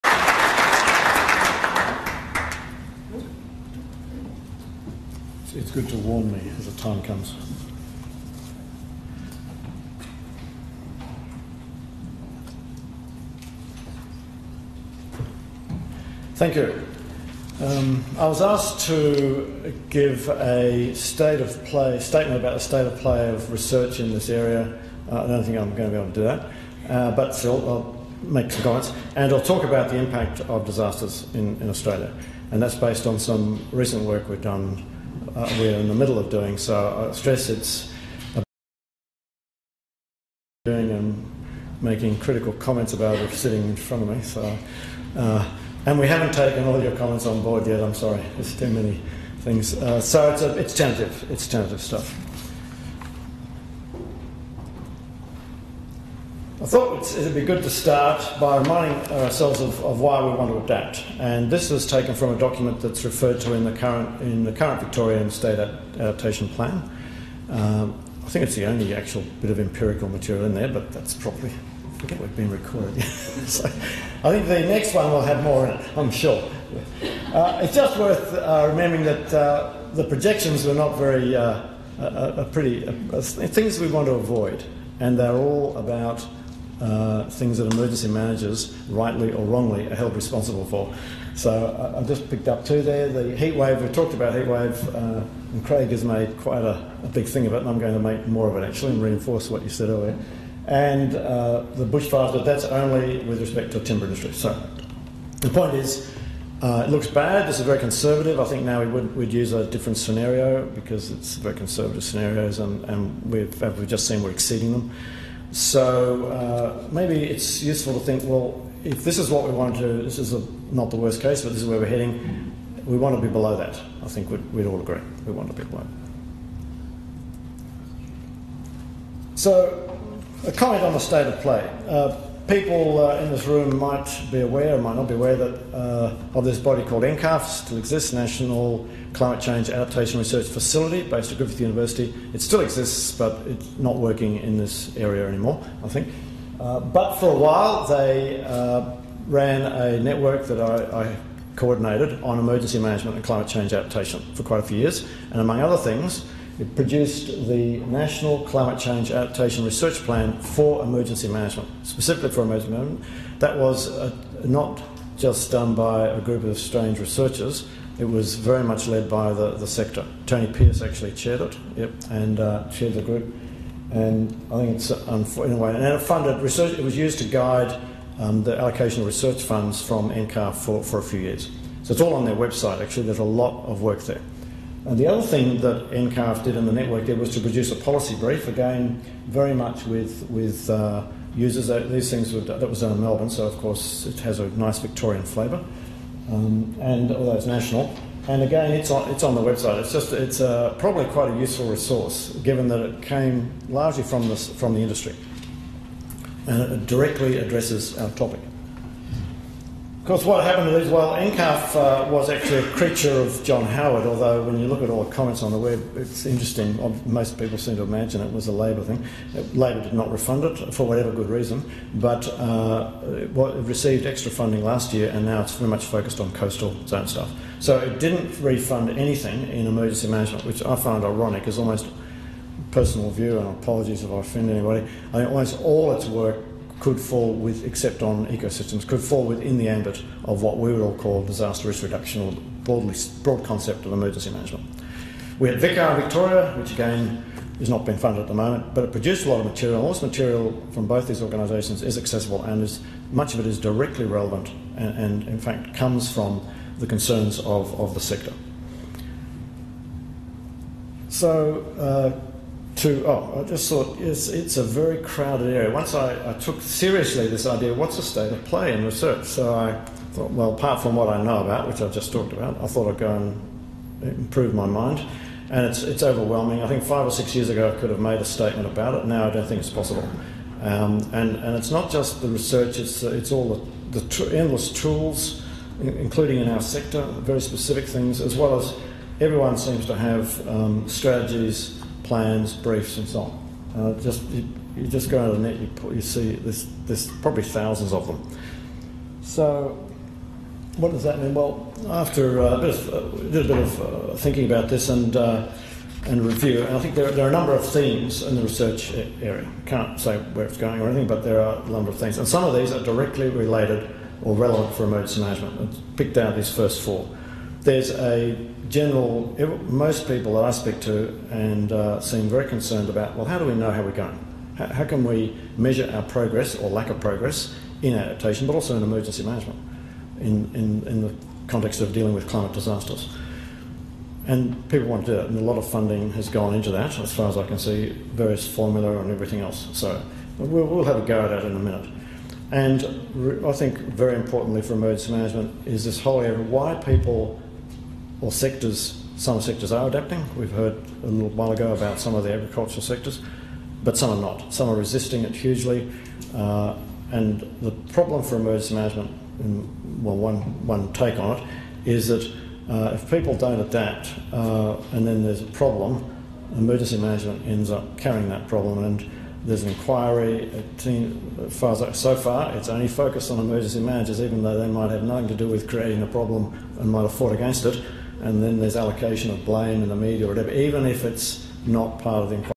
CFA Climate Change Forum 2015 - RMIT
CFA Climate Change Forum 2015 - Hearing from experts RMIT John Handmer

Tue, Jul 14 2015